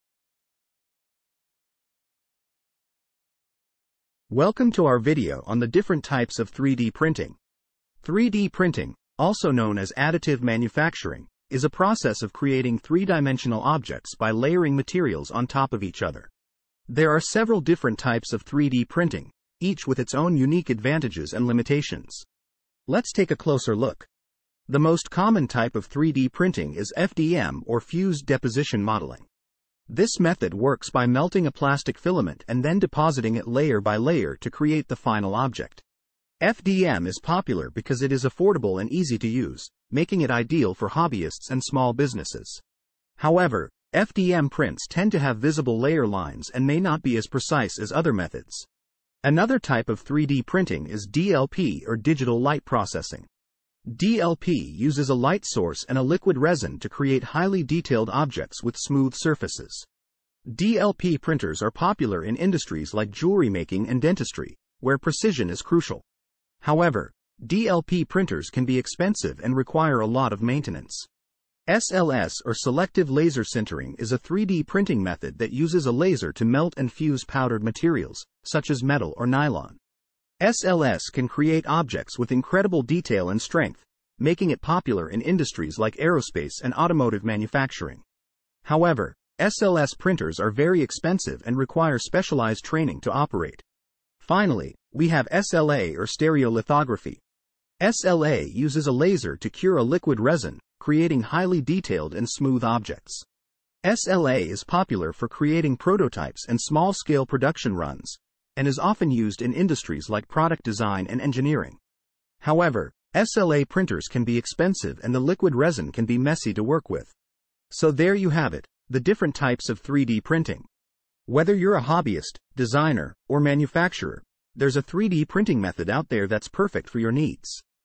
Exploring the Different Types of 3D Printers: Which One is Right for You?
In this video, we dive into the world of 3D printing and explore the various types of 3D printers available, including FDM, DLP, SLS, and SLA. We discuss the unique advantages and limitations of each type, and provide insights into the industries and applications that each is best suited for. Whether you're a hobbyist, designer, or manufacturer, this video will help you determine which type of 3D printer is right for your needs.